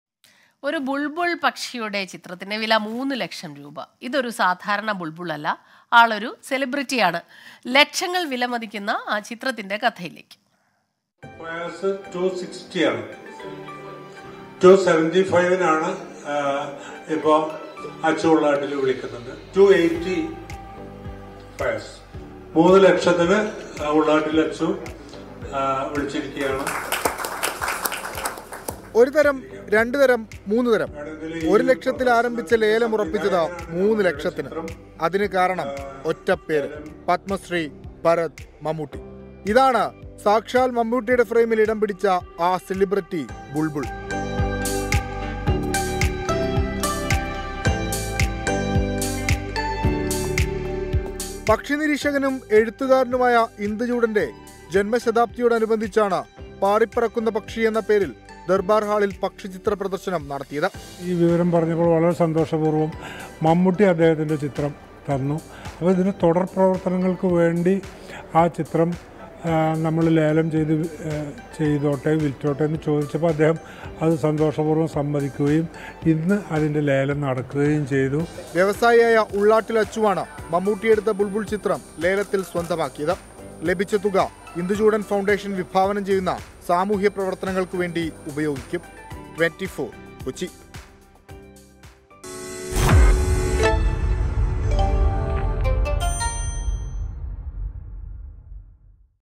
മമ്മൂട്ടി പകർത്തിയ ബുൾബൾ പക്ഷിയുടെ ചിത്രം വിറ്റത് ലക്ഷങ്ങൾക്ക്..!
Mammootty's picture of bulbul bird sold for lakhs..!